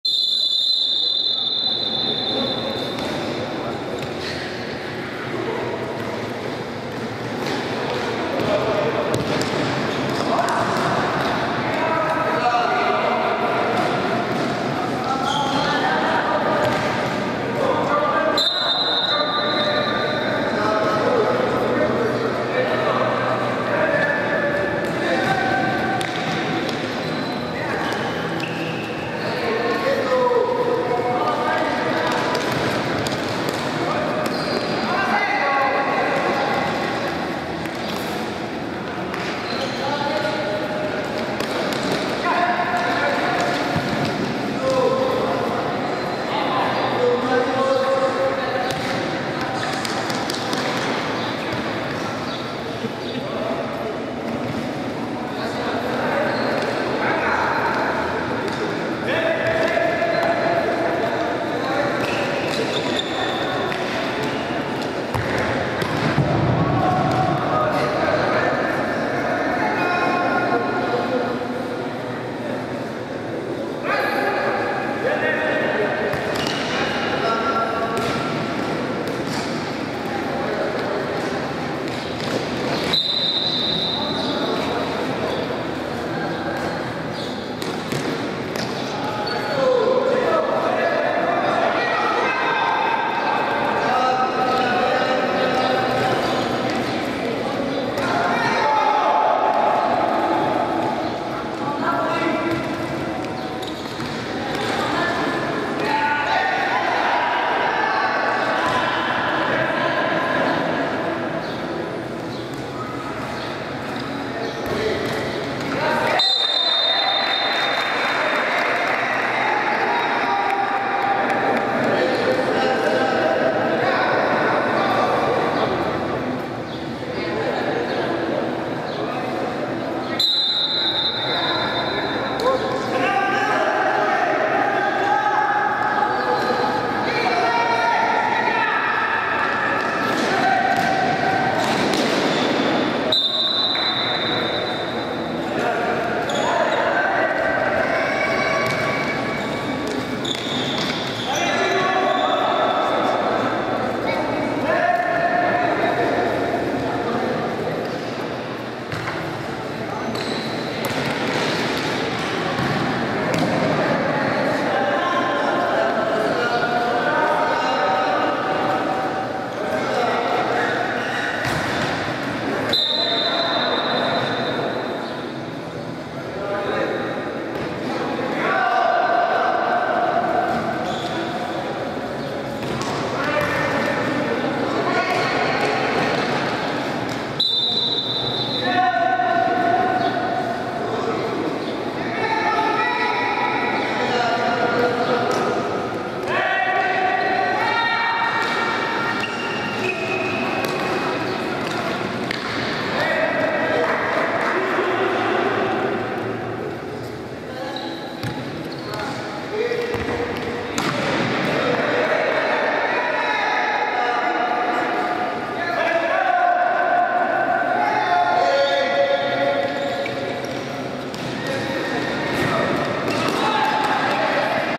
Deportivo Ajpop se impuso a Nápoli
Patzún, en el marco del fútbol sala de este municipio, Deportivo Ajpop se impuso por marcador de 6-9 ante Nápoli.  El próximo fin de semana se espera la definición del equipo que pasará a la final.